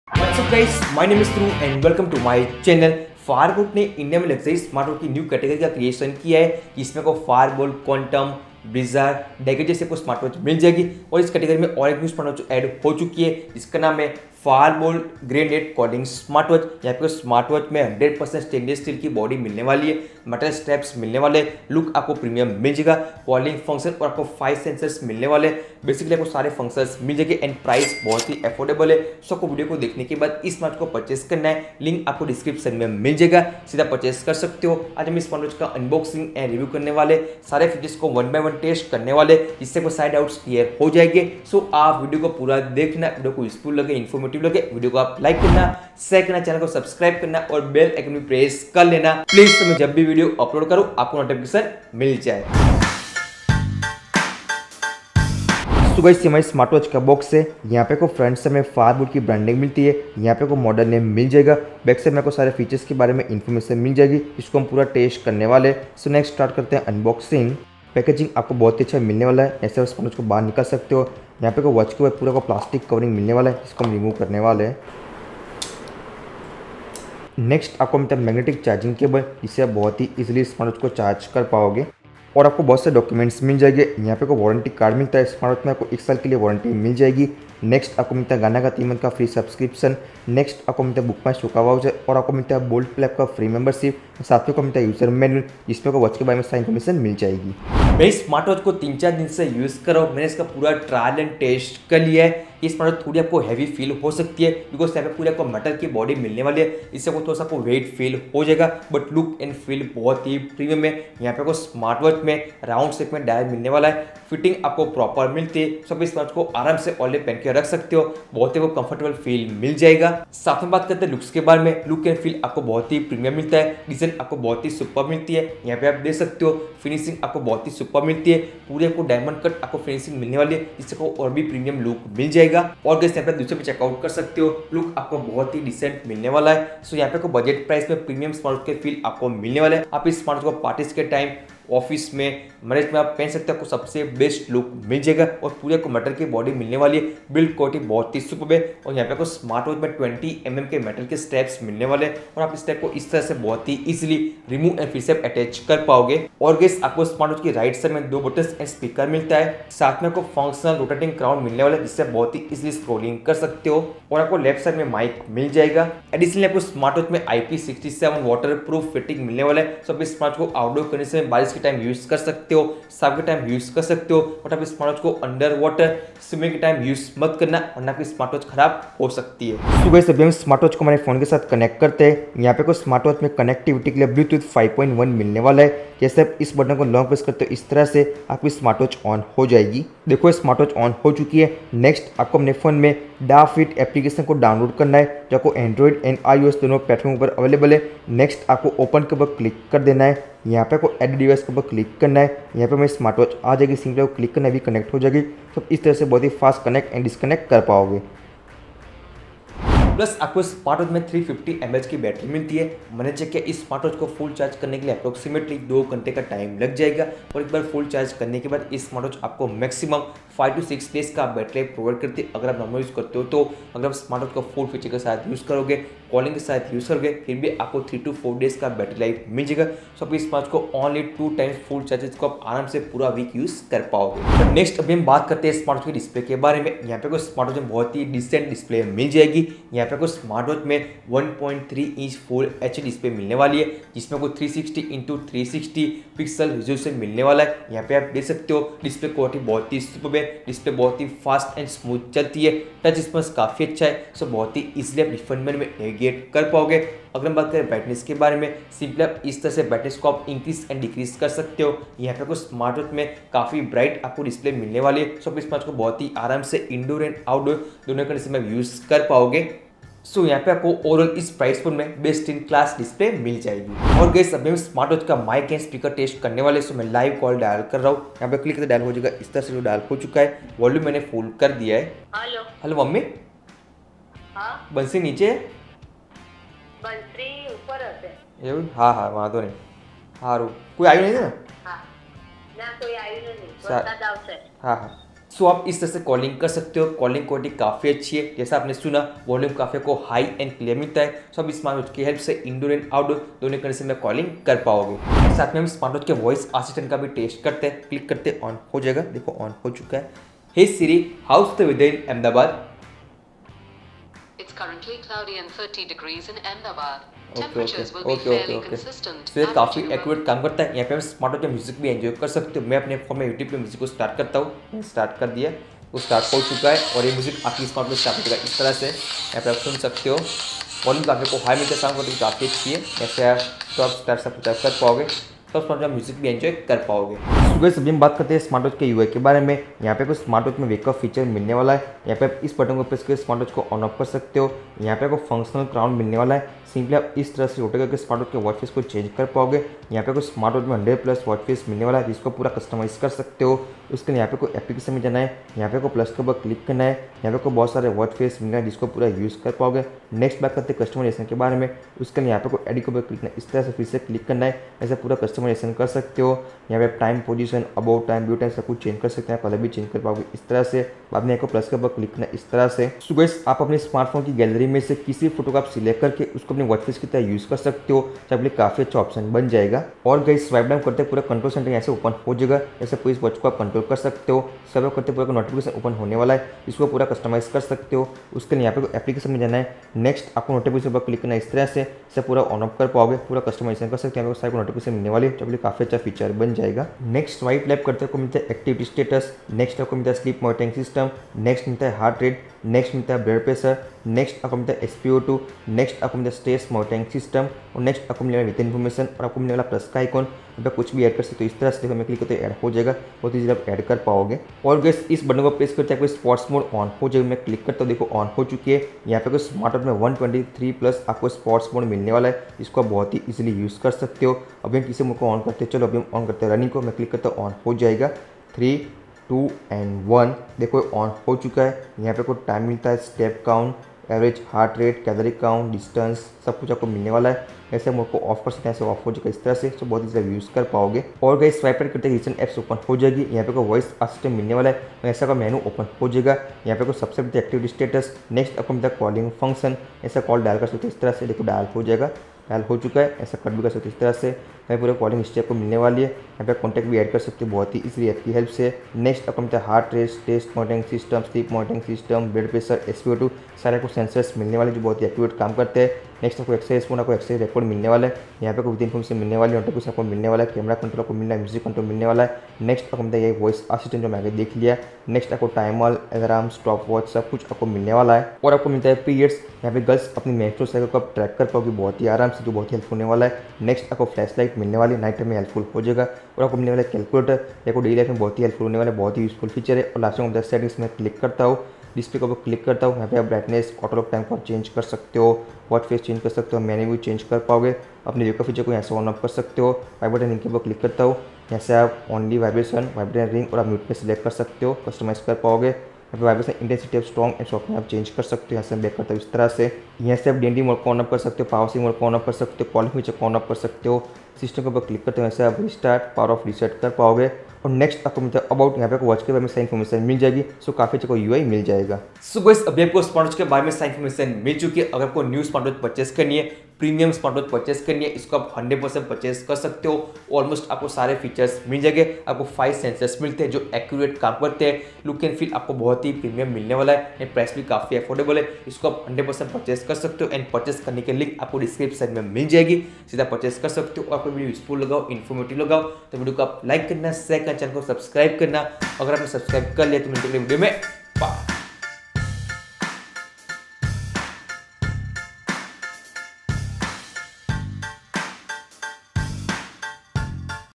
Fire boltt Grenade Calling Smartwatch | Fire boltt Grenade Unboxing and Review | Rugged Smartwatch
Buying Link -

0:00 - Intro
0:55 - Unboxing
1:37 - Review
2:59 - How To Connect Fire boltt Grenade
3:29 - Battery Test
3:54 - Display
4:33 - Calling Test
5:37 - User Interface
8:42 - Outro

( Join For Discounted Price Tech Product)